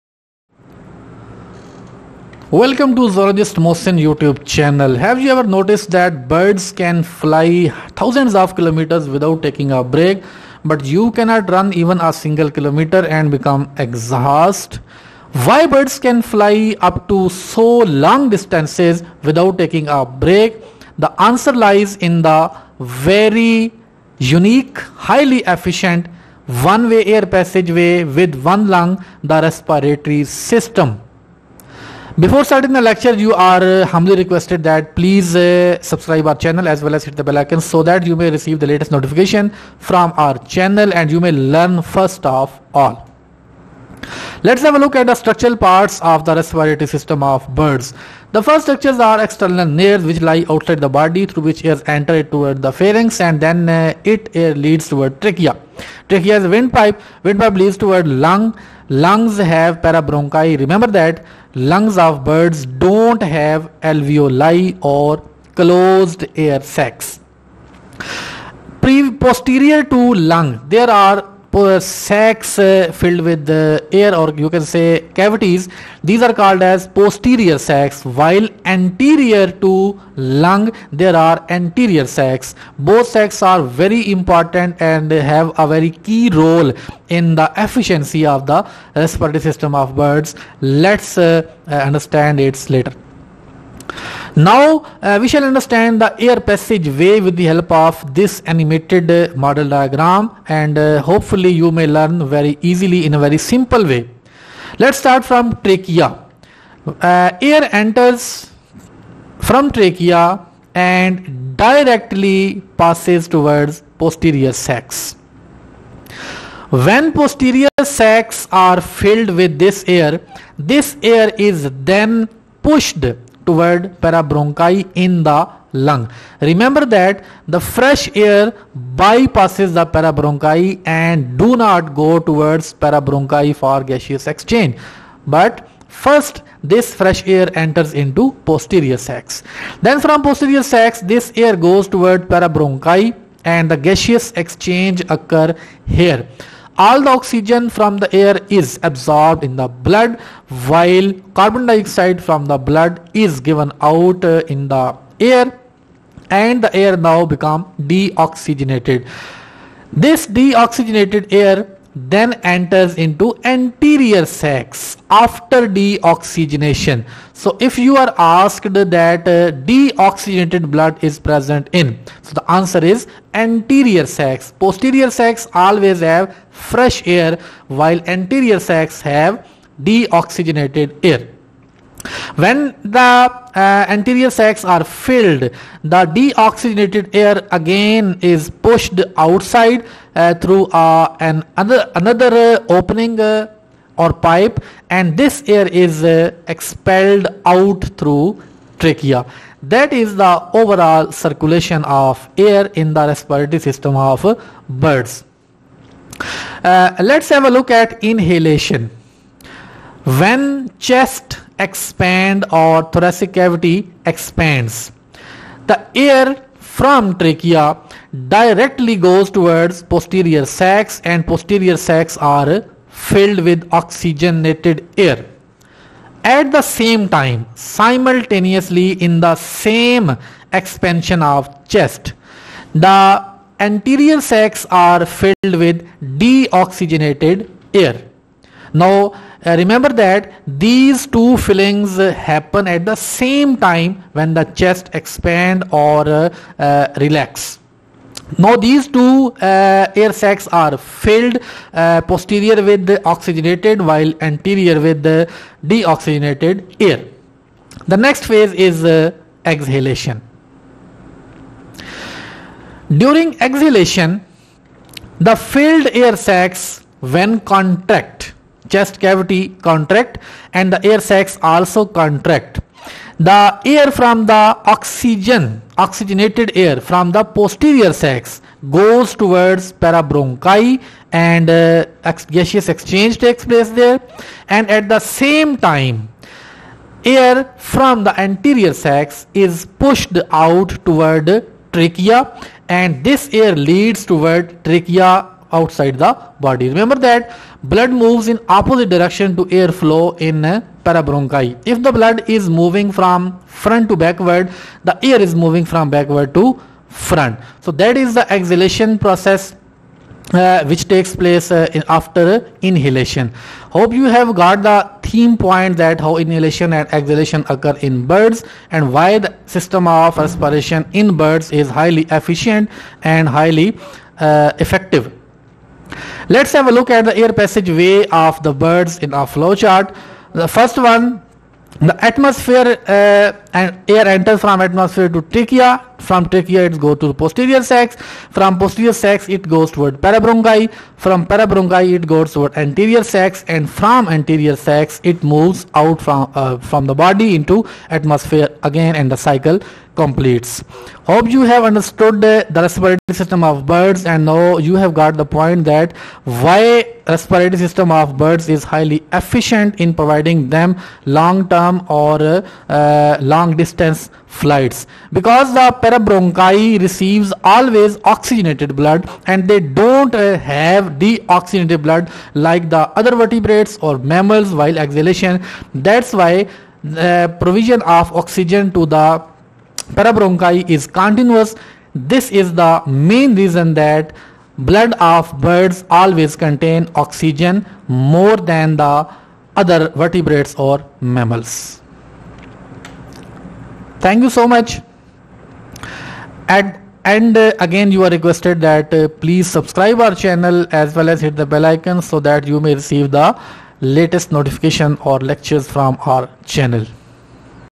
Respiratory System of Birds | Air Flow in Birds | Animated Lecture
Why birds can fly long distances without fatigue or tiring? The Answer is Respiratory system. Bird Respiratory system has few unique features which enable their efficiency and capability. How air flows in birds? How air is continuously supplied to parabronchi? What is role of sacs in Birds Breathing. All these questions are answered in this lecture with the help of air flow animation in birds respiratory system.